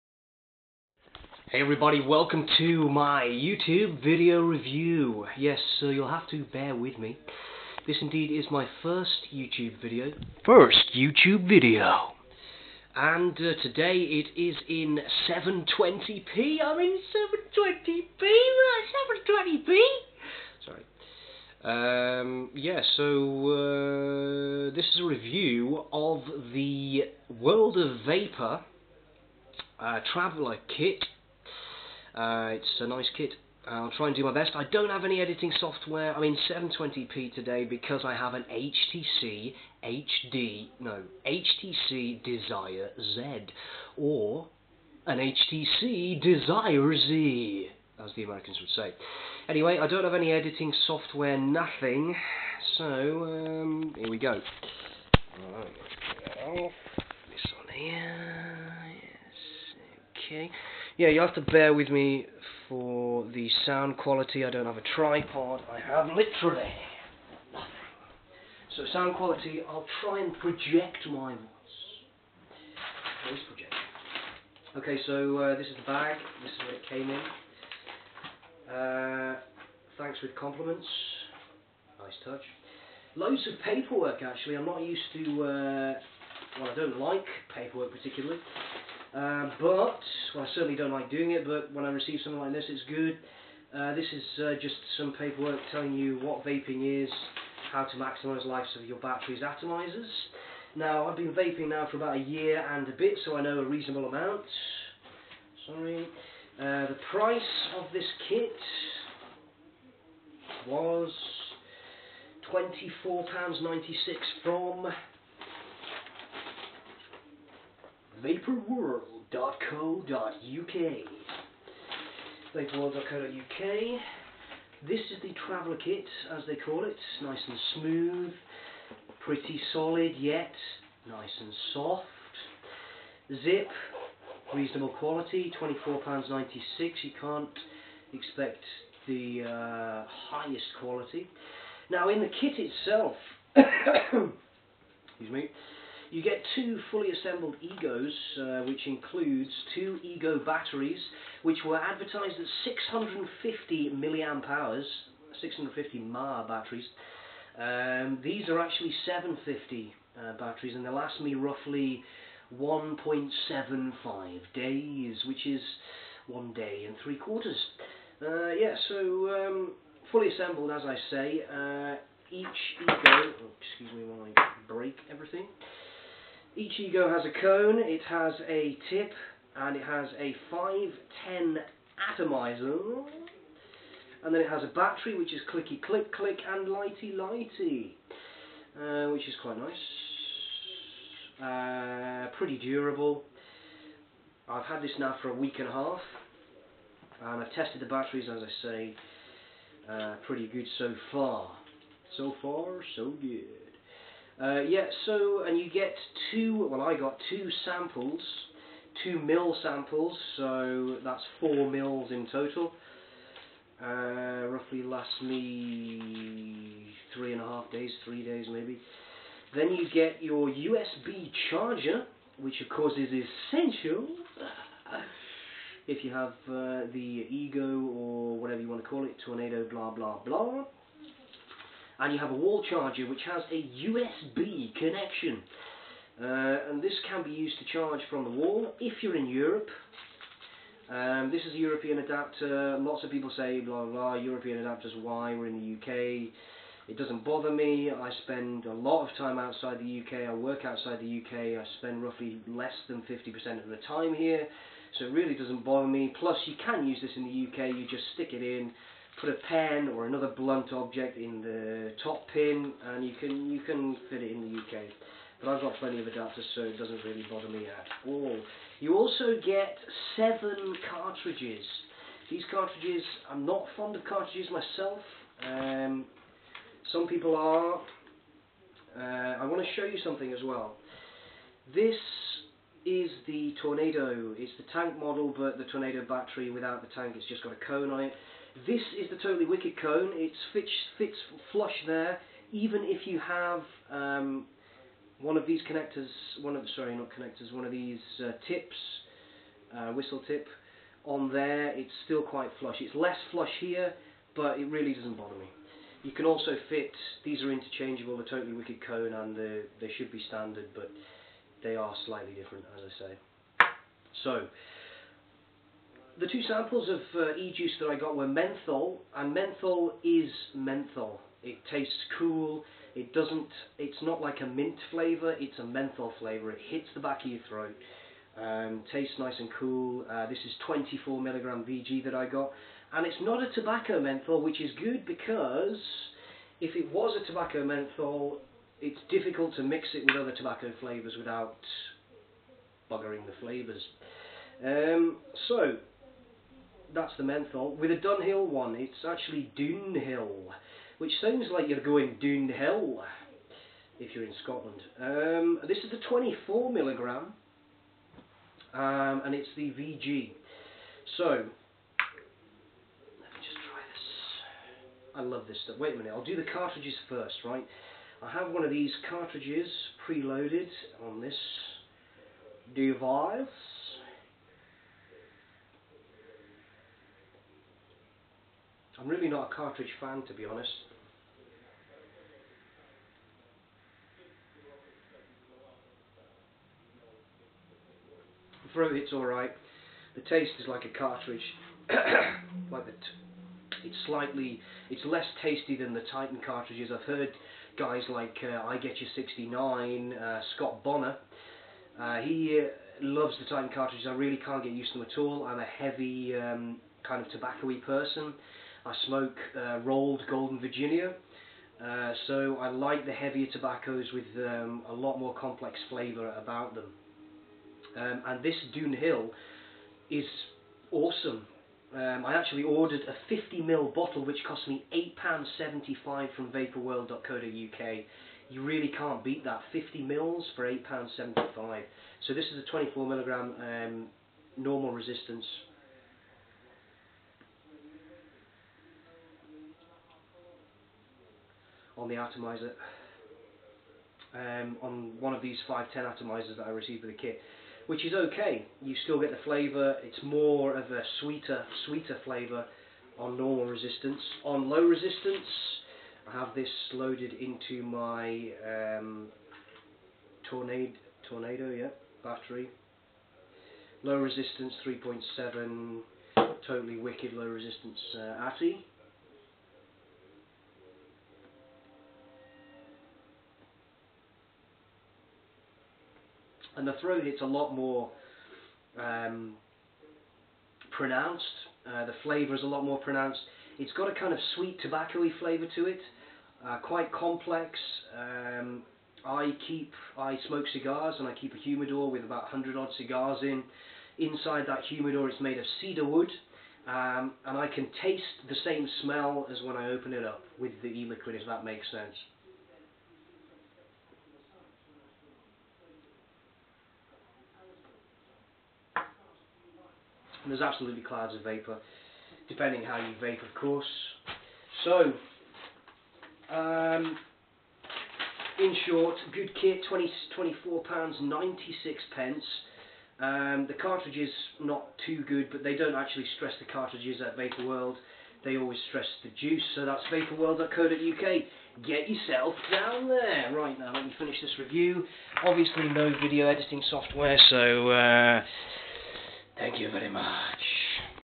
Vapourworld PowerVaper Traveller Kit & Dunehill Ejuice Review
Quick review of the vapourworld traveller kit (not theworldofvapour as I said at the start-ooops) - also this is advertised as having 6 carts, I got 7 :) and advertised with 650mAh batteries, the manual says 750mAh and the batteries in the kit recieved seem to have a longer life than the standard 650mAh tornado from TW.  This is the PowerVaper 510 traveller kit and worldofvapour Dunehill tobacco flavour from DeKang (sweet, ceder woody goodness!).

This is an emerging company and their prices are the most competitive I have ever seen.  All prices quoted minus VAT @ 20%.  Prices may be variable and are quoted at the time of review (16 Mar 2011).

I saw this kit on one of the Tamworth UK vape mini meet videos starring, among others, the legendary; S2V; Hifistud and THV- thanks for highlighting this great deal.

The primary reason that I conducted this review is because the company's prices are excellent and their service is the same, (and to test my neeeew smartphone! :) Seriously though,  I recieved this package in a day and a half!

I have no video editing software so the vid is what it is, apologies for the lack of interesting cut scenes and closeups :(  The vid quality shld be awesome but the sound is suitably rubbish (phone mic) :(